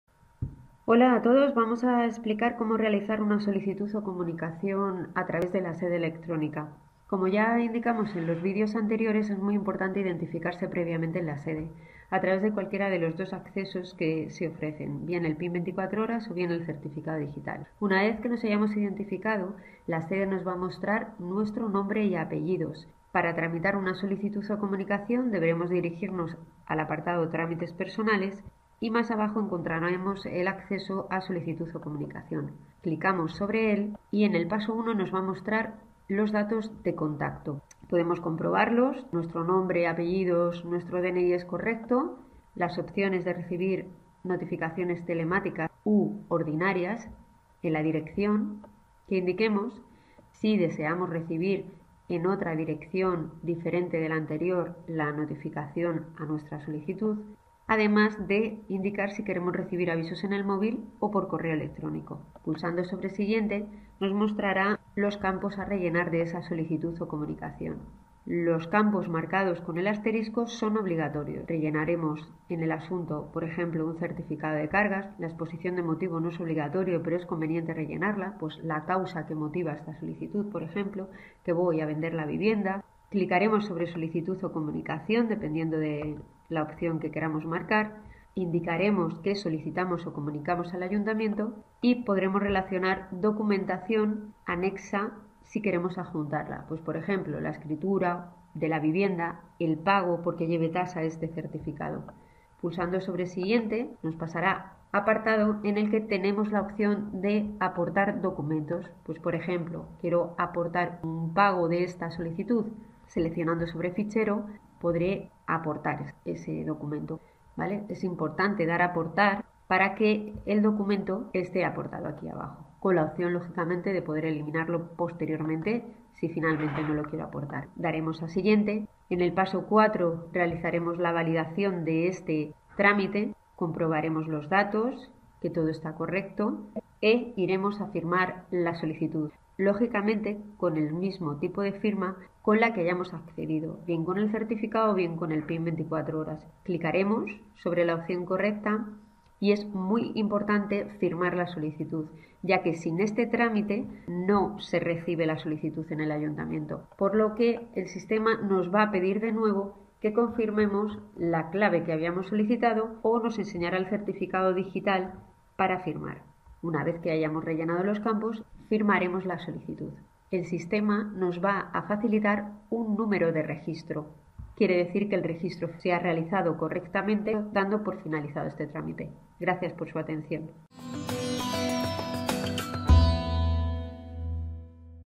Video tutorial: Solicitud o comunicación en Sede Electrónica
Aprende a realizar una Solicitud o una comunicación en la Sede Electrónica de Moralzarzal